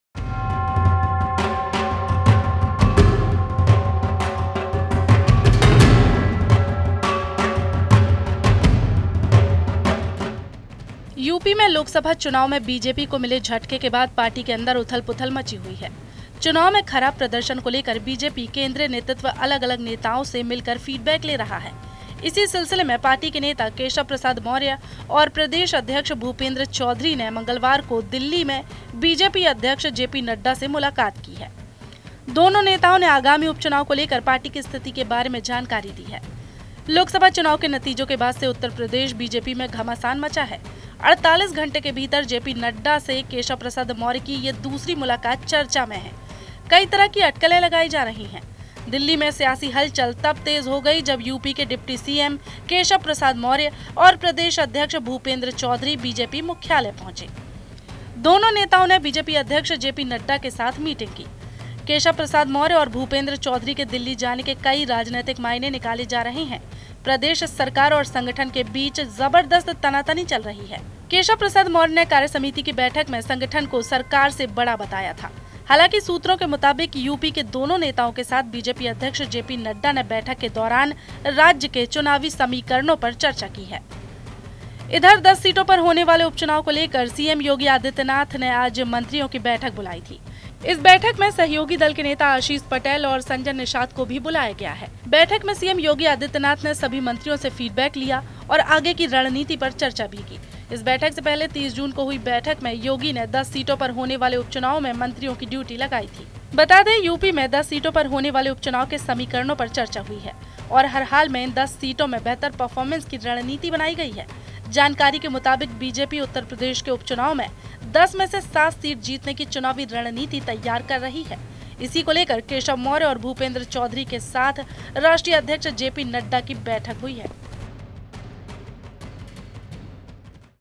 यूपी BJP में सियासी हलचल, Keshav Maurya और Bhupendra Chaudhary ने JP Nadda से क्यों की मुलाकात?
यूपी में लोकसभा चुनाव में बीजेपी को मिले झटके के बाद पार्टी के अंदर उथल-पुथल मची हुई है. चुनाव में खराब प्रदर्शन को लेकर बीजेपी केंद्रीय नेतृत्व अलग-अलग नेताओं से मिलकर फीडबैक ले रहा है. इसी सिलसिले में पार्टी के नेता केशव प्रसाद मौर्य और प्रदेश अध्यक्ष भूपेंद्र चौधरी ने मंगलवार को दिल्ली में बीजेपी अध्यक्ष जेपी नड्डा से मुलाकात की है.  दोनों नेताओं ने आगामी उपचुनाव से लेकर पार्टी की स्थिति के बारे में जानकारी दी है.

There is turmoil within the party after the setback faced by BJP in the Lok Sabha elections in UP. The BJP central leadership is meeting different leaders and taking feedback regarding the poor performance in the elections. In this connection, party leader Keshav Prasad Maurya and state president Bhupendra Chaudhary met BJP President JP Nadda in Delhi on Tuesday.  Both the leaders have given information about the situation of the party regarding the upcoming by-elections.